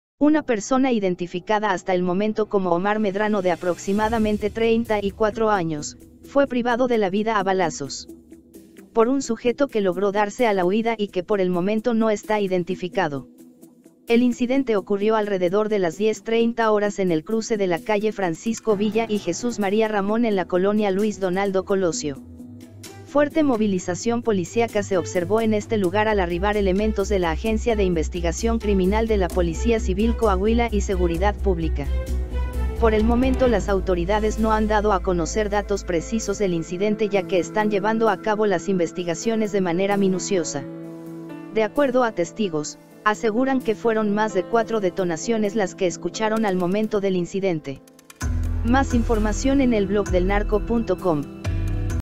Ejecutan a hombre a balazos en Acuña, Coahuila
Acuña, Coah. – Una persona identificada hasta el momento como Omar Medrano de aproximadamente 34 años, fue privado de la vida a balazos.

Por un sujeto que logró darse a la huida y que por el momento no está identificado.  El incidente ocurrió alrededor de las 10:30 horas en el cruce de la calle Francisco Villa y Jesús María Ramón en la colonia Luis Donaldo Colosio.  Fuerte movilización policiaca se observó en este lugar al arribar elementos de la Agencia de Investigación Criminal de la Policía Civil Coahuila y Seguridad Pública.